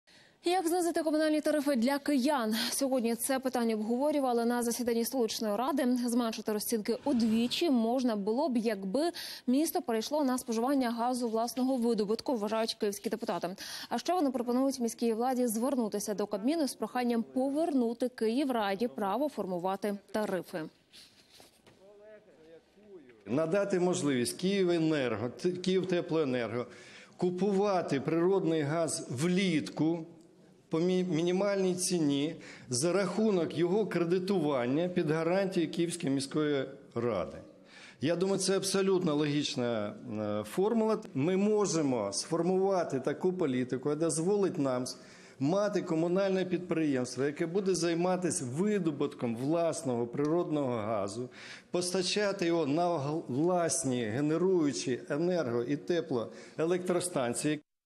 В "Опозиційній платформі - За життя" пропонують вдвічі знизити комунальні тарифи для киян
Як знизити комунальні тарифи для киян? Сьогодні це питання обговорювали на засіданні столичної ради